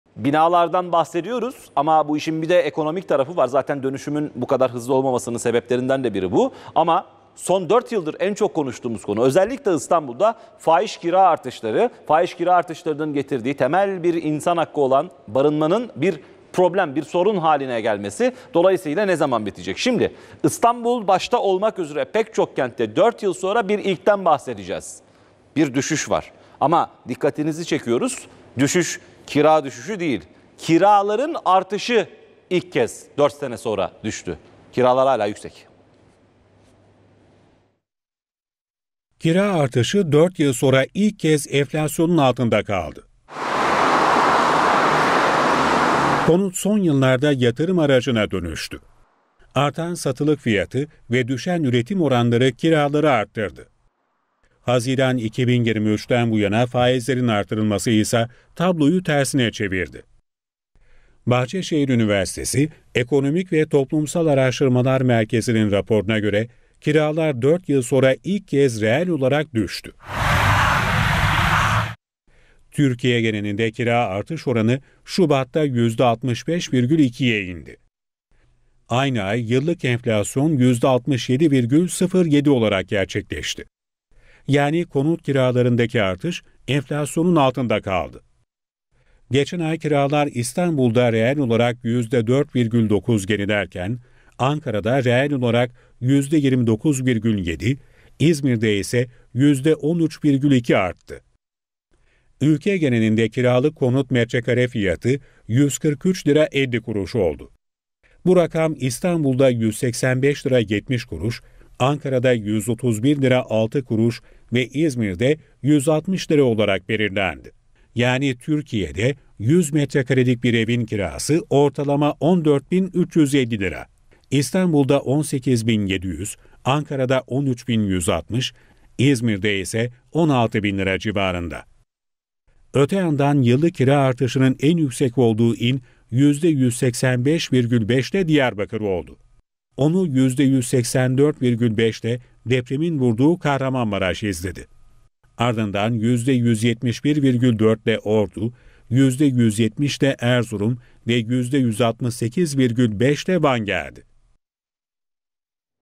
Kiralarda 4 Yıl Sonra İlk Düşüş! | NTV
Kira Artışı 4 Yıl Sonra İlk Kez Enflasyonun Altında Kaldı

Kira artışları yıllardır gündemden düşmüyor. Artış, 4 yıl sonra ilk kez, Şubat ayında enflasyonun altında kaldı. Yani kiralar reel olarak geriledi. Büyükşehirlerde en büyük düşüş İstanbul'da yaşandı.